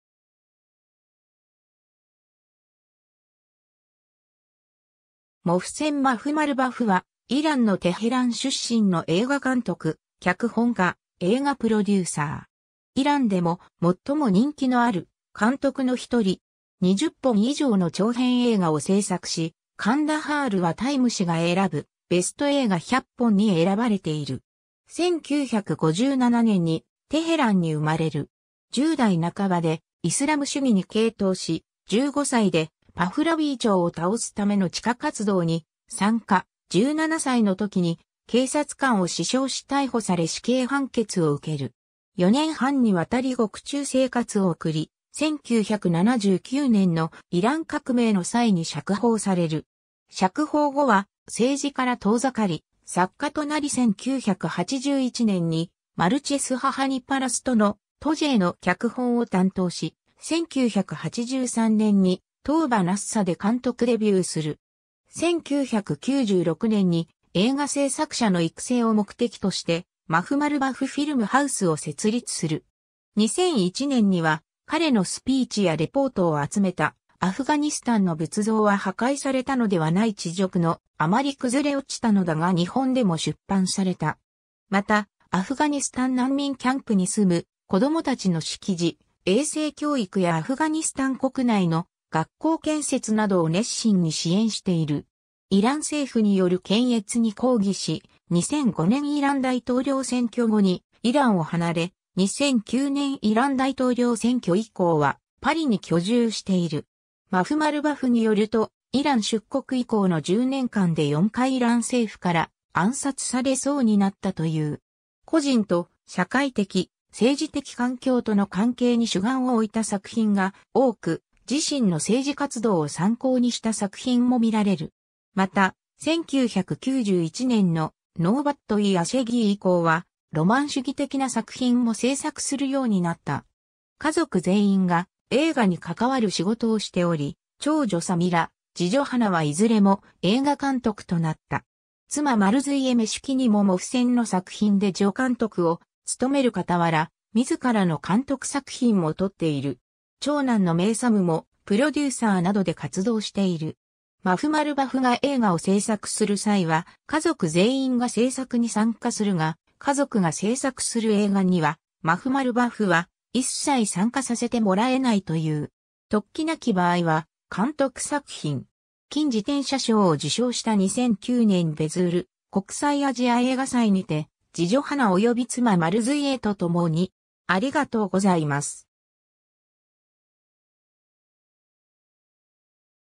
モフセン・マフマルバフ
モフセン・マフマルバフ（محسن مخملباف, ラテン文字転写：Mohsen Makhmalbaf, 1957年5月29日 - ）は、イランのテヘラン出身の映画監督・脚本家・映画プロデューサー。
イランでも最も人気のある監督の1人。
20本以上の長編映画を制作し、『カンダハール』はタイム誌が選ぶ「ベスト映画100本」に選ばれている。
 1957年にテヘランに生まれる。
10代半ばでイスラム主義に傾倒し、15歳でパフラヴィー朝を倒すための地下活動に参加、17歳の時に警察官を刺傷し逮捕され死刑判決を受ける。
4年半に渡り獄中生活を送り、1979年のイラン革命の際に釈放される。
釈放後は政治から遠ざかり、作家となり1981年にマルチェス・ハハニパラストの『Towjeeh』の脚本を担当し、1983年に『Tawba Nasuh』で監督デビューする。
1996年に映画製作者の育成を目的として、マフマルバフ・フィルム・ハウスを設立する。
 2001年には彼のスピーチやレポートを集めた『アフガニスタンの仏像は破壊されたのではない恥辱のあまり崩れ落ちたのだ』が日本でも出版された。
また、アフガニスタン難民キャンプに住む子供達の識字・衛生教育やアフガニスタン国内の学校建設などを熱心に支援している。
 イラン政府による検閲に抗議し、2005年イラン大統領選挙後にイランを離れ、2009年イラン大統領選挙以降はパリに居住している。
マフマルバフによると、イラン出国以降の10年間で4回イラン政府から暗殺されそうになったという。
 個人と社会的・政治的環境との関係に主眼を置いた作品が多く、自身の政治活動を参考にした作品も見られる。
また、1991年の『Nobat e Asheghi』以降はロマン主義的な作品も製作するようになった。
 家族全員が映画に関わる仕事をしており、長女サミラ、次女ハナはいずれも映画監督となった。
妻マルズィエ・メシュキニもモフセンの作品で助監督を務めるかたわら、自らの監督作品も撮っている。
長男のメイサムもプロデューサーなどで活動している。
マフマルバフが映画を製作する際は家族全員が製作に参加するが、家族が製作する映画にはマフマルバフは一切参加させてもらえないという。
 特記なき場合は監督作品。
 金自転車賞を受賞した2009年ヴェズール国際アジア映画祭にて、次女ハナ（左）および妻マルズィエ（中）とともに